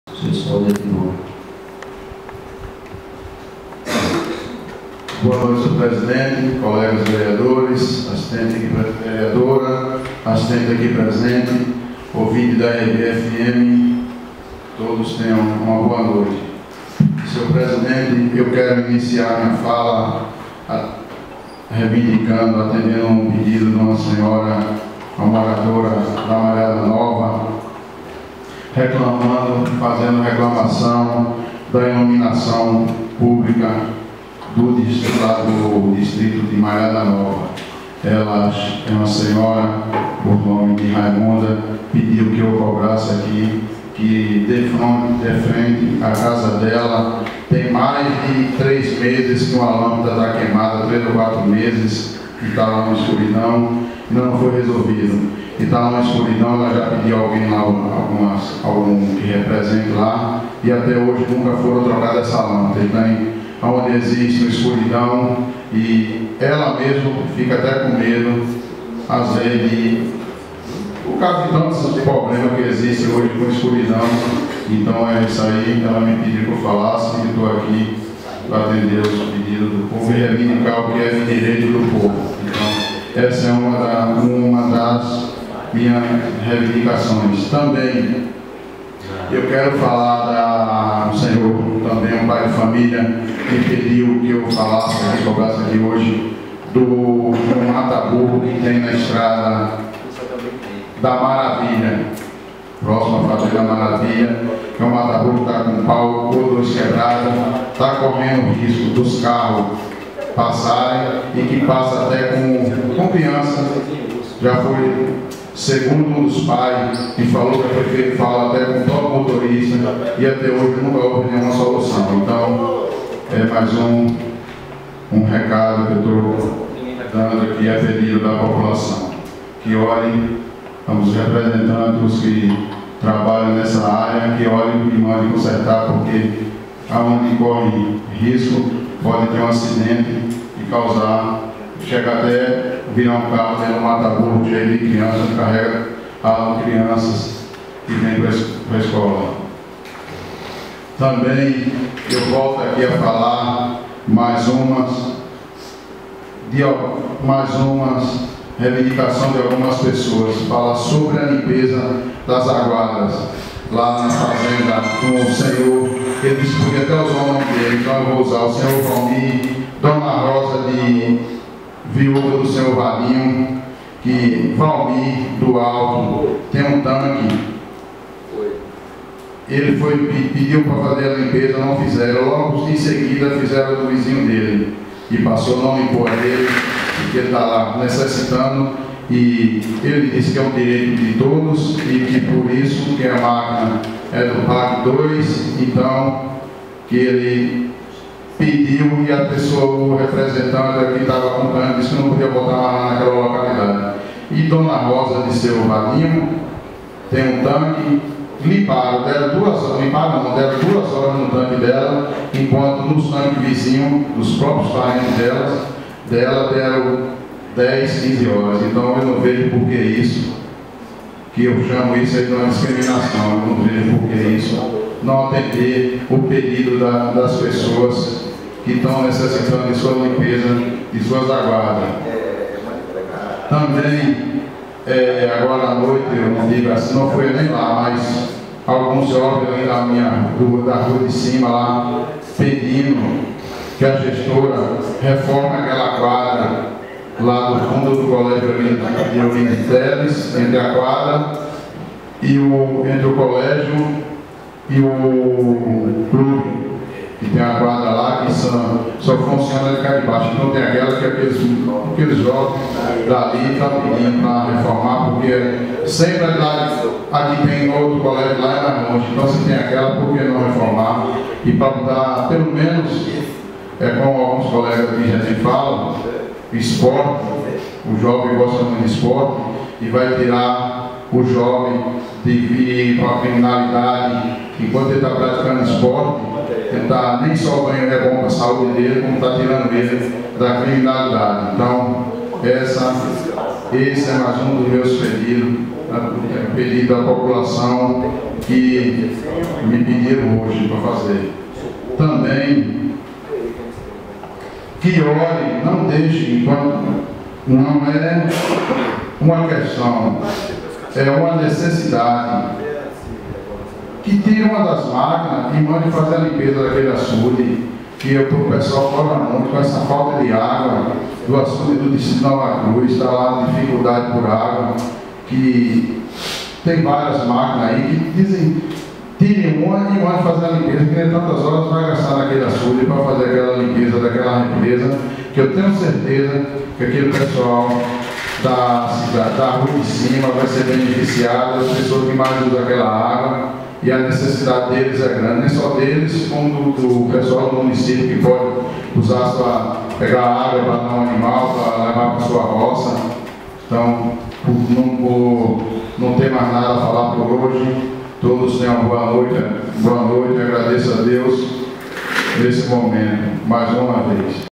Isack Soares (PSD) sessão em 25 de outubro de 2018
O vereador Isack Soares (PSD) cobrou igualdade nas limpezas de águadas.